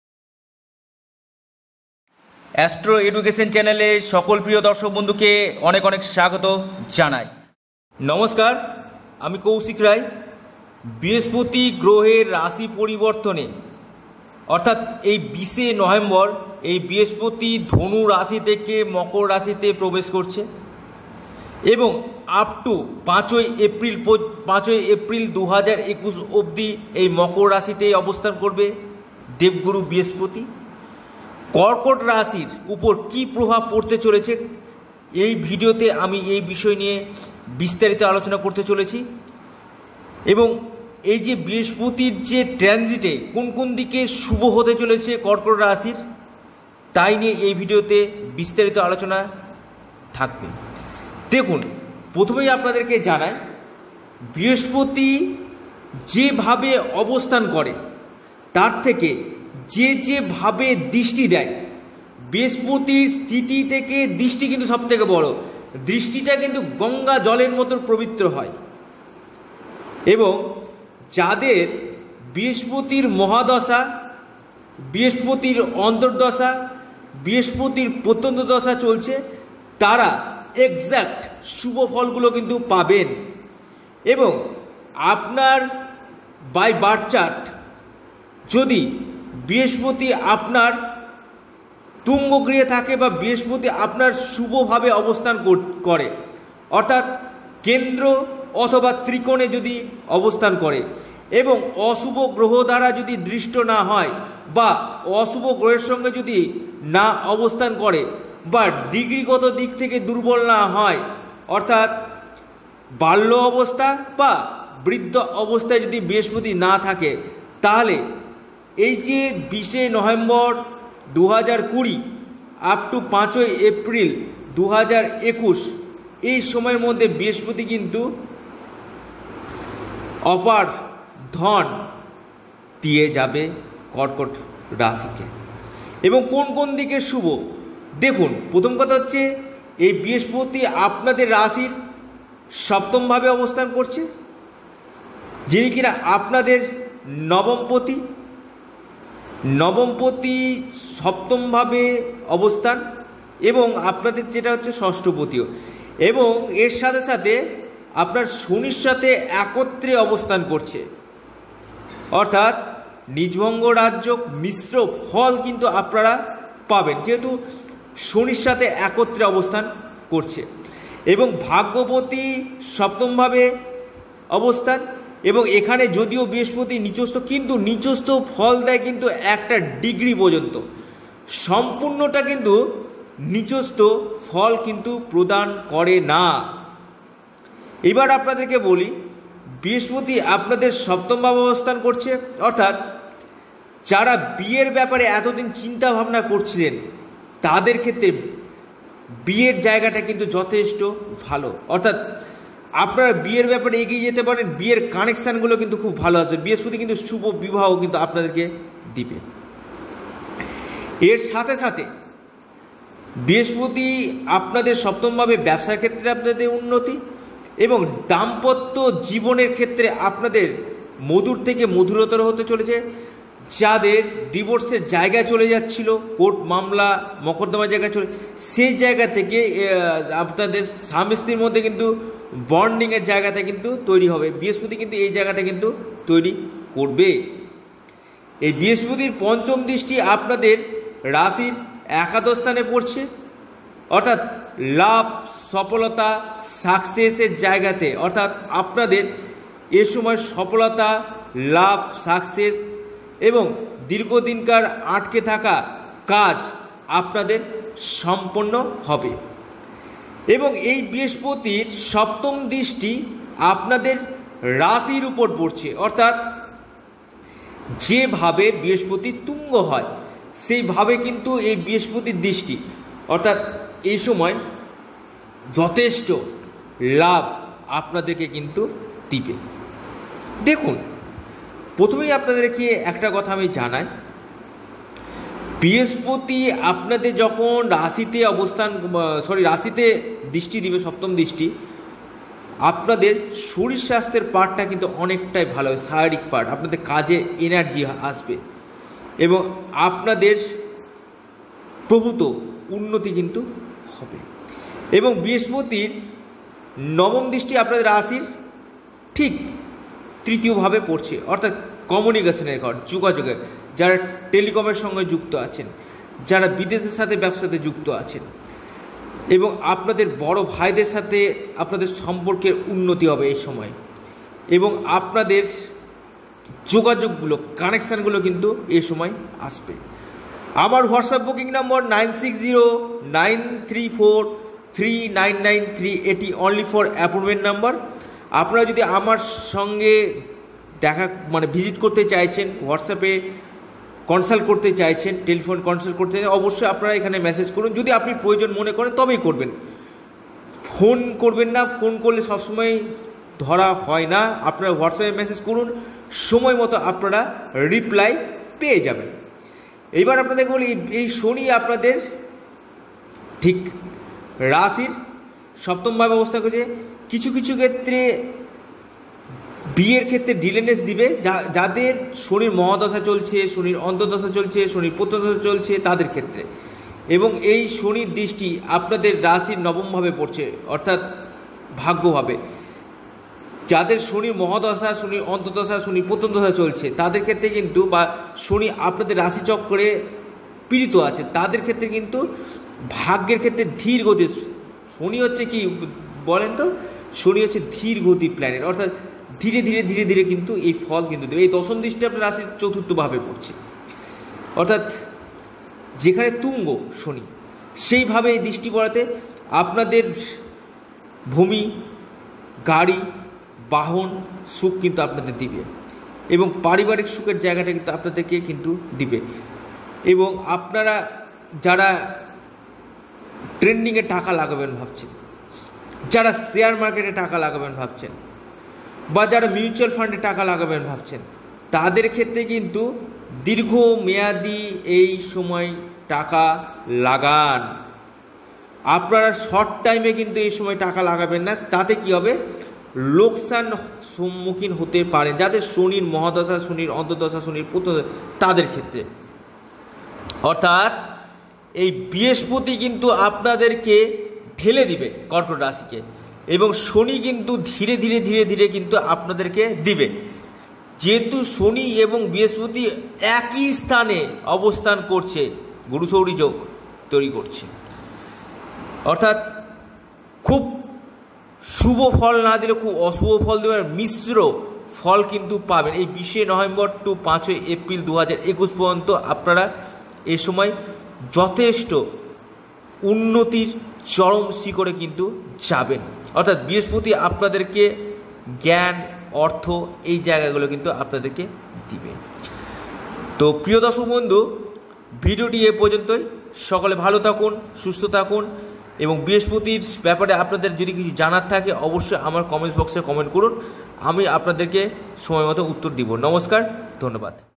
বৃহস্পতির রাশি পরিবর্তন কর্কট রাশির উপর কি প্রভাব!!
বৃহস্পতি গ্রহের রাশি পরিবর্তনে কী প্রভাব পড়বে কর্কট রাশির উপর এই বিষয় নিয়ে বিস্তারিত আলোচনা করা হয়েছে ধন্যবাদ!